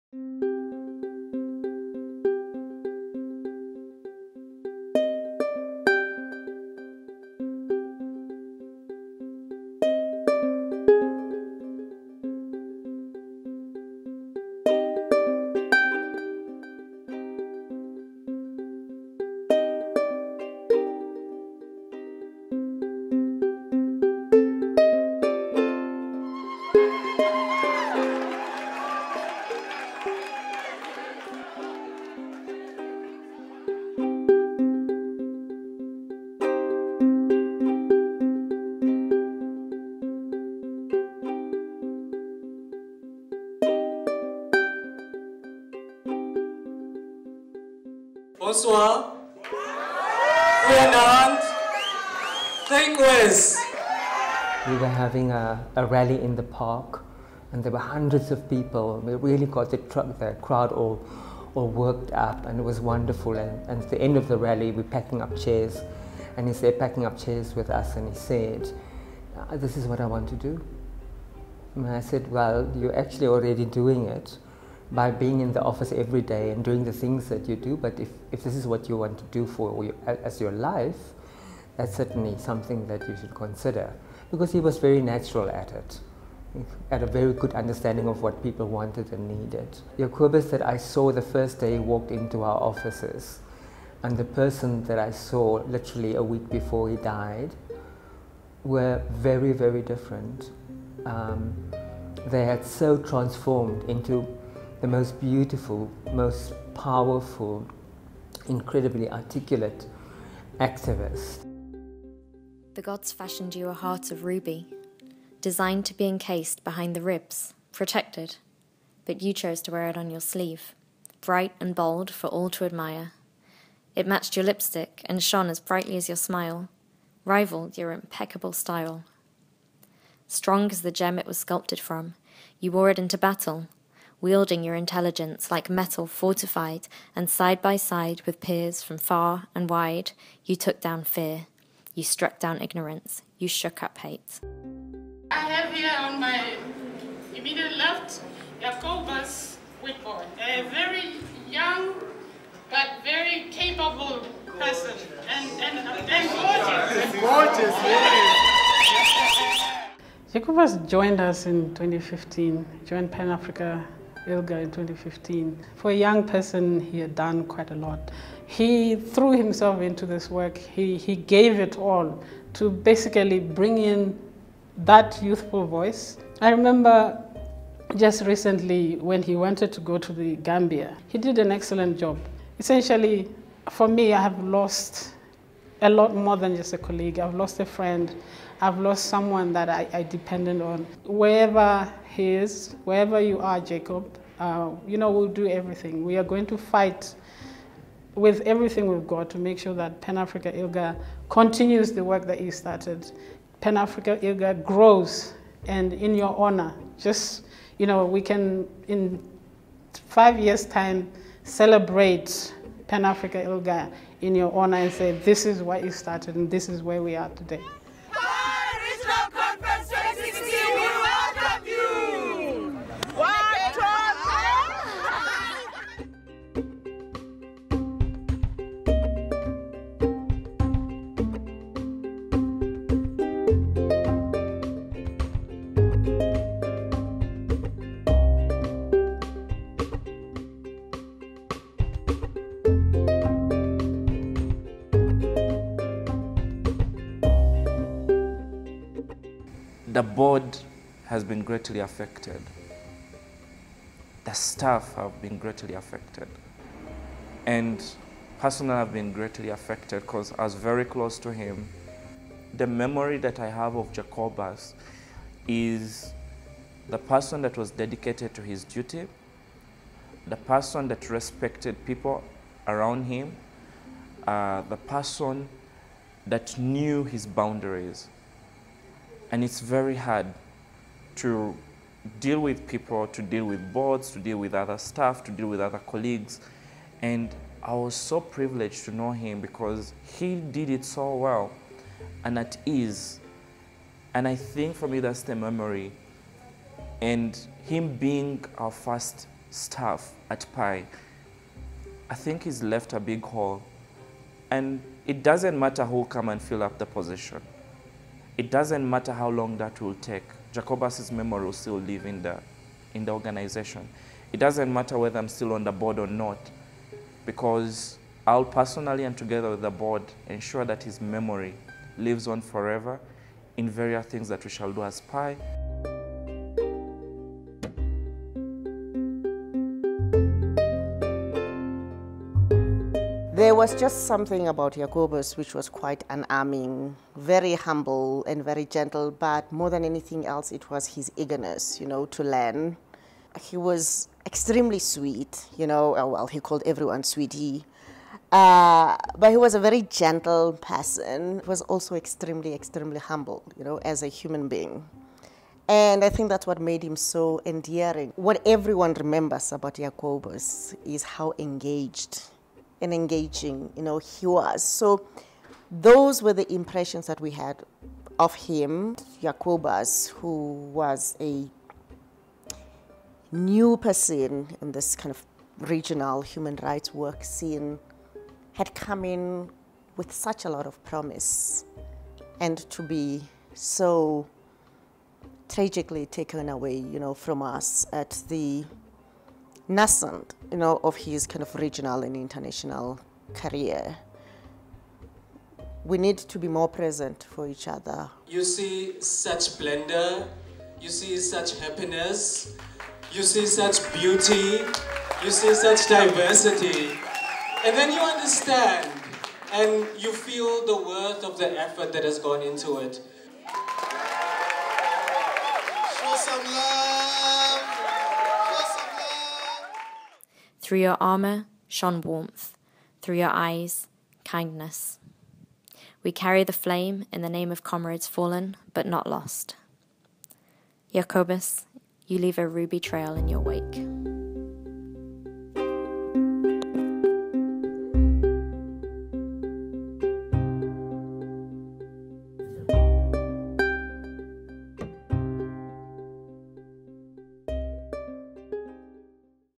Jacobus Witbooi
Jacobus Witbooi, 16 December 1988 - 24 November 2016.  Jacobus was the African Coordinator for Pan Africa ILGA at the time of his death.  A resilient young LGBTIQ activist, born in Namibia moved to South Africa as PAI's first employee.  Sadly, he contracted malaria while he attended the African Commission on Human and Peoples Rights in the Gambia.  He is sorely missed by a community across that loved him dearly.  RIP Jacobus.  We will all continue to build on your legacy.